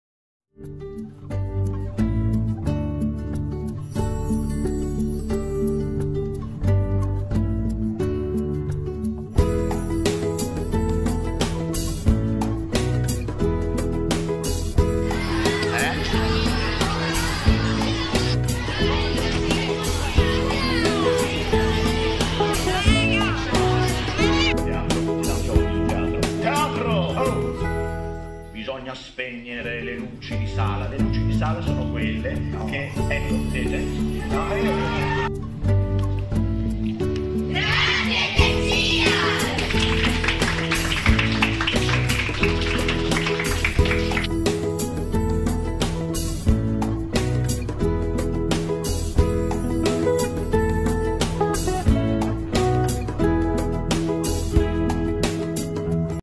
Invita una classe a teatro 2017 - Tecsial
Quest’anno il Maggio all’infanzia ha festeggiato la ventesima edizione: una ricorrenza significativa per il più importante festival dedicato alle arti per l’infanzia del Sud Italia.

Alla luce del successo della passata edizione, Teatri di Bari ha riproposto l’iniziativa Invita una classe a teatro, che ha dato la possibilità a 35 classi di assistere gratuitamente agli spettacoli in programma per il Festival, grazie al contributo delle aziende del territorio.

Grazie al contributo di Tecsial, il 30 maggio 2017,  la 1^B dell'Istituto Biagio Grimaldi ha  potuto assistere gratuitamente allo spettacolo Tutti insieme con Romeo e Giulietta, in scena al Teatro Abeliano per il Maggio all'Infanzia.